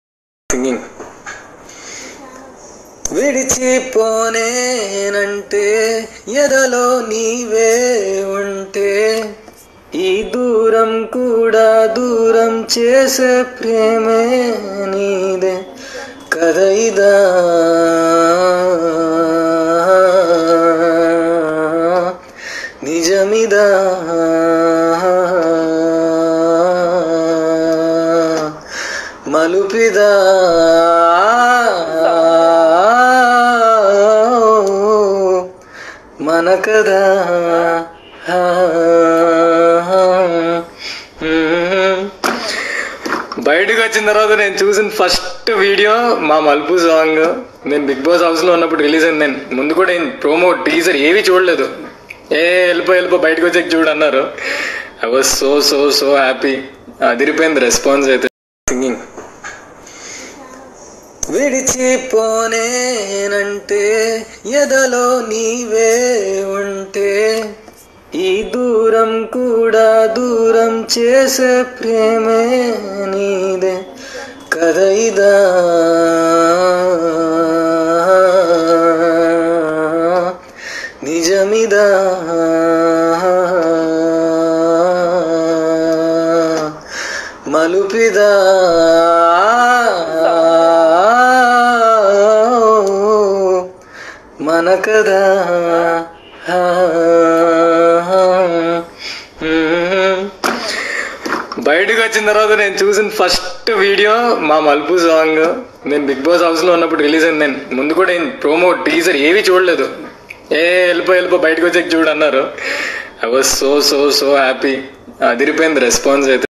పాట పడుతూ ఏడ్చేశాడు భయ్యా | Shanmukh Jaswanth gets emotional while singing | MANATFI
Welcome to Mana TFI TV Channel
Telugu latest movie teasers, trailers, fun videos and exclusive interviews are available in our channel.